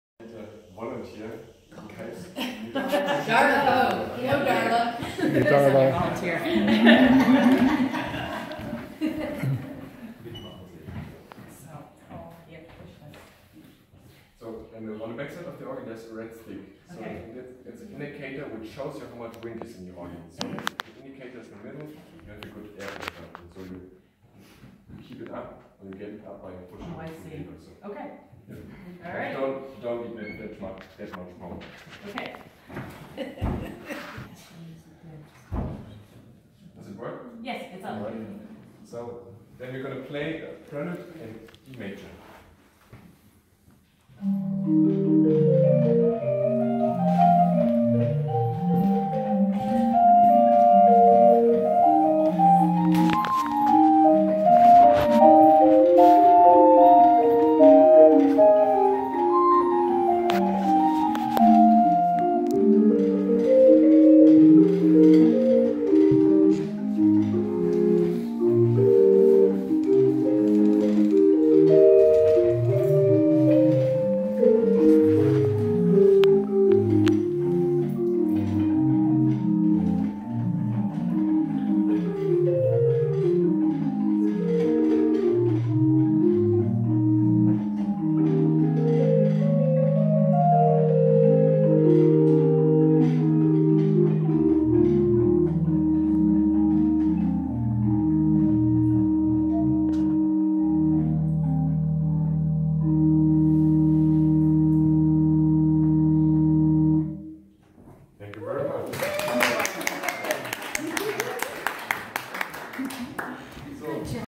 Bach House in Eisenach, Germany
As part of Luther 500 tour we visited the Bach House in Eisenach Germany. A member of our group was asked to help power the pipe organ.